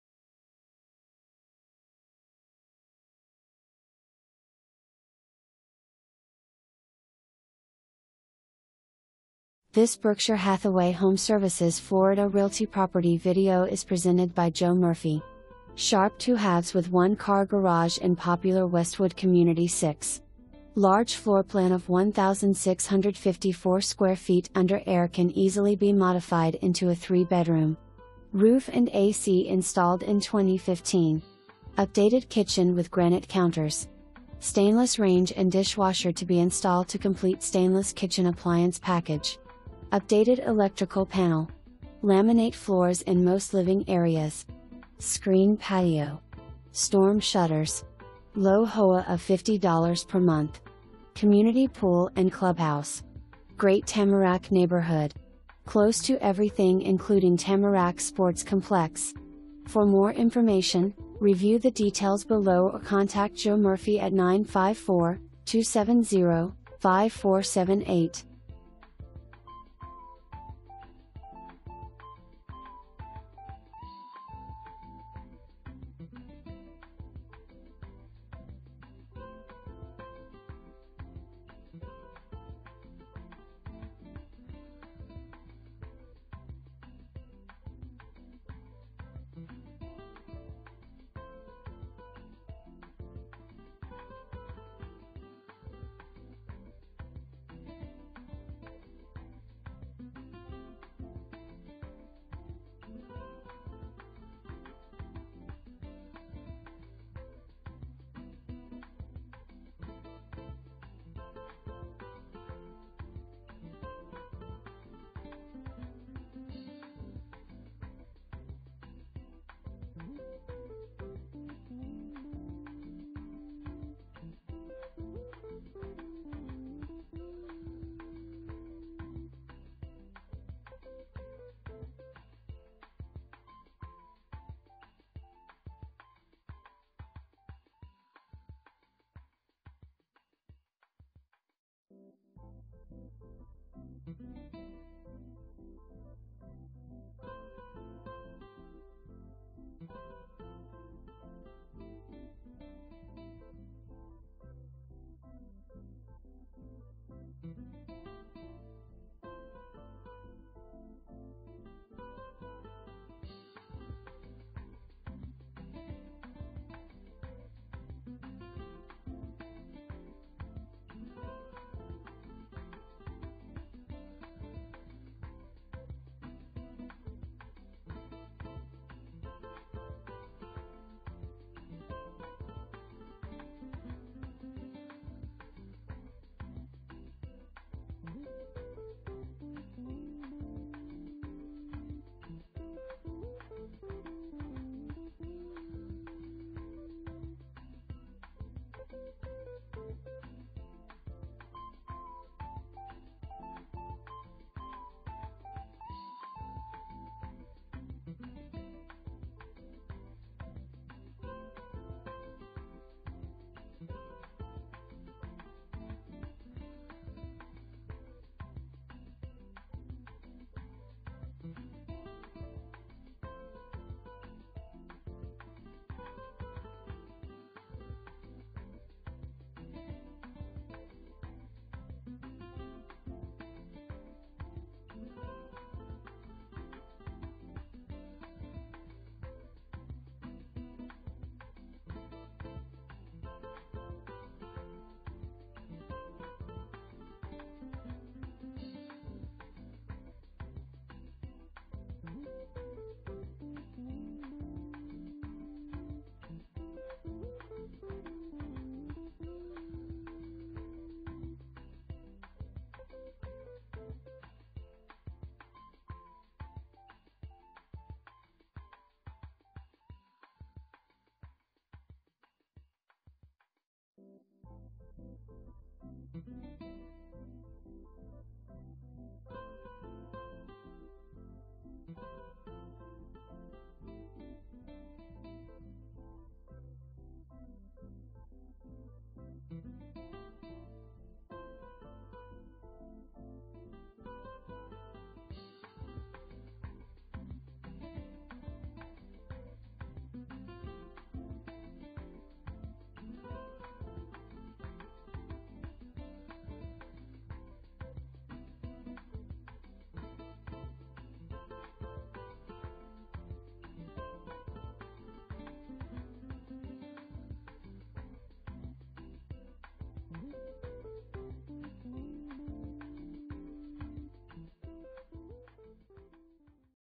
Berkshire Hathaway HomeServices Florida Realty - 8012 NW 101st Ave
$435,000, 2.00 bath, 1,654 SF, MLS# F10362179

Sharp 2/2 with 1 car garage in popular Westwood Community 6. Large floorplan of 1,654 Sq Ft under air can easily be modified into a 3 bedroom. Roof and A/C installed in 2015. Updated kitchen with granite counters. Stainless range and dishwasher to be installed to complete stainless kitchen appliance package. Updated electrical panel. Laminate floors in most living areas. Screen patio. Storm shutters. Low HOA of $50 per month. Community pool and clubhouse. Great Tamarac neighborhood! Close to everything including Tamarac Sports Complex.

Joe Murphy
Berkshire Hathaway HomeServices Florida Realty